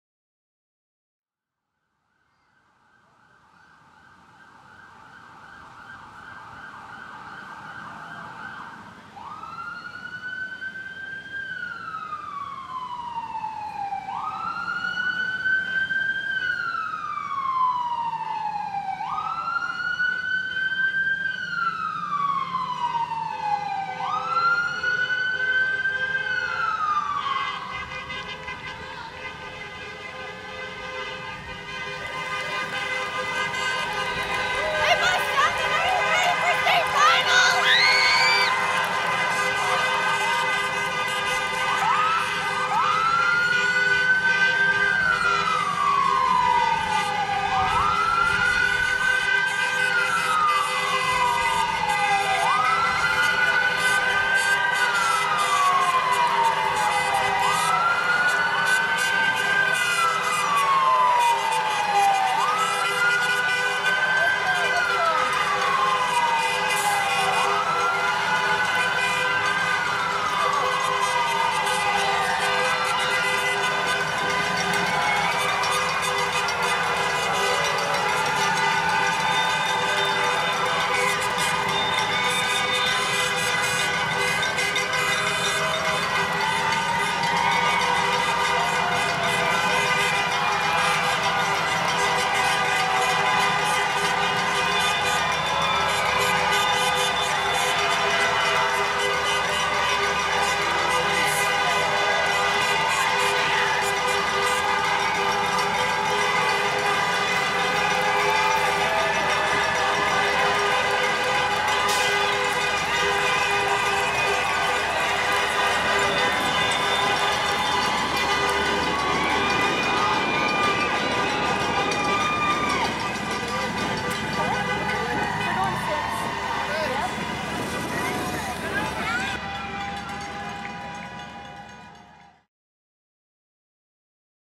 2016 State Finals Sendoff
What do you get with 9 busses, carrying 300+ kids, 2 trucks, 1 van/trailer a police escort and a historic town square?

State Finals Sendoff!